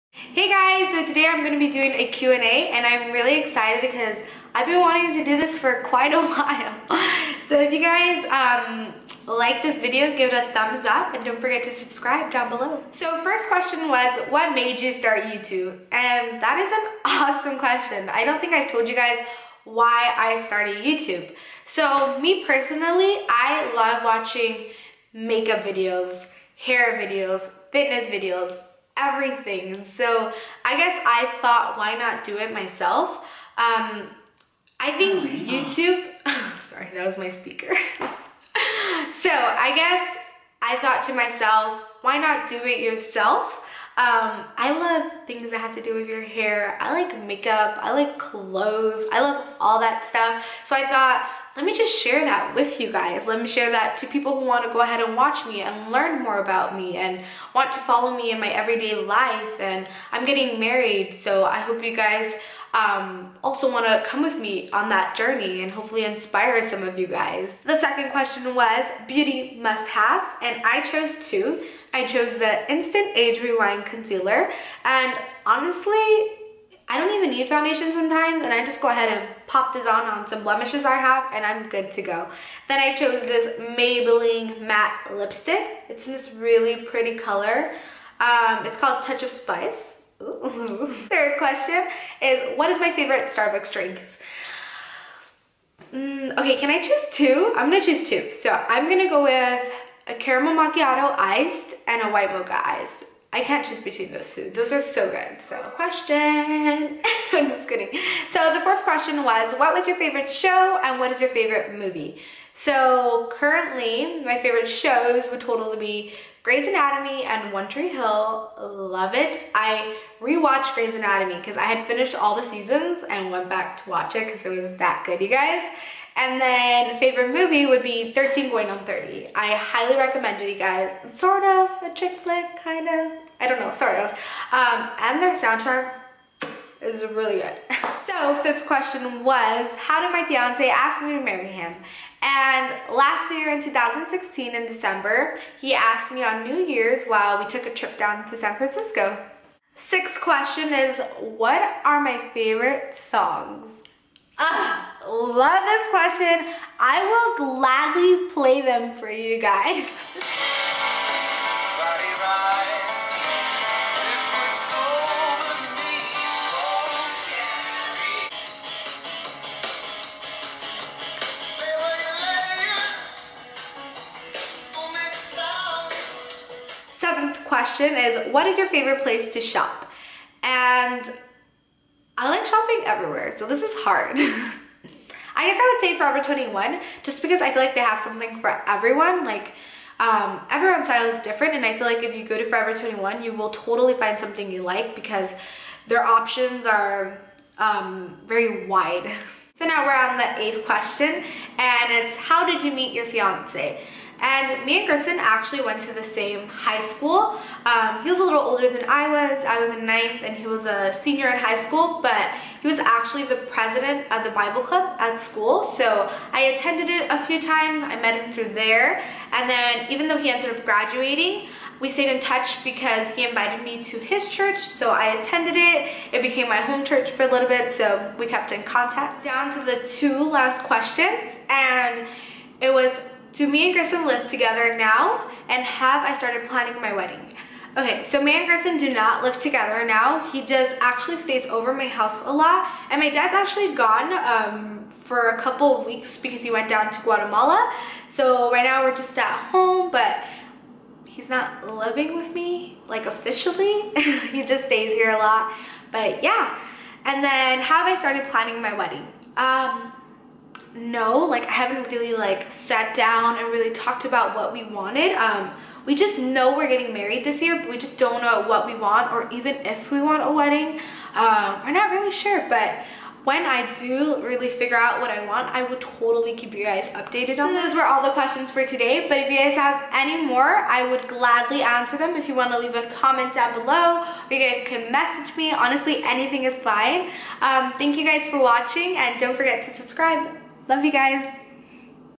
Q&A/Learn More About Me
Guys I've been wanting to do a Q&A since I started my channel but I wanted the questions to come from YOU. Now for the pass two months I've gotten a few and here I am answering them. Hope you enjoyed learning a little more about me❤️ Also don't be afraid to ask a little more!